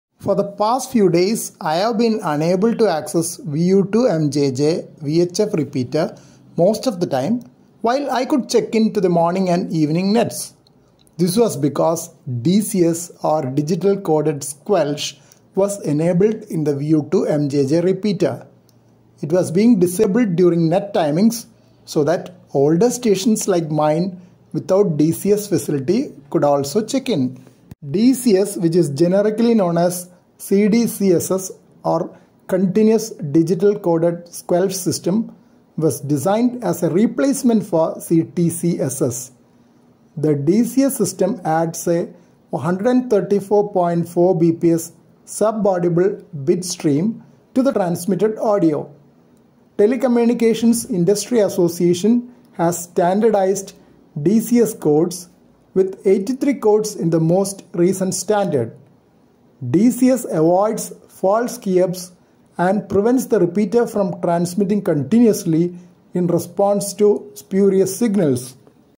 What is DCS or Digital-Coded Squelch in Ham Radio?
For the past few days I have been unable to access VU2MJJ VHF repeater most of the time, while I could check in to the morning and evening nets. This was because DCS or Digital-Coded Squelch was enabled in the VU2MJJ repeater. It was being disabled during net timings so that older stations like mine without DCS facility could also check in!

DCS which is generically known as  CDCSS or Continuous Digital-Coded Squelch System, was designed as a replacement for CTCSS. The DCS systems adds a 134.4 bps sub-audible bitstream to the transmitted audio. Telecommunications Industry Association has standardized DCS codes with 83 codes in the most recent standard. DCS avoids false keyups and prevents the repeater from transmitting continuously in response to spurious signals.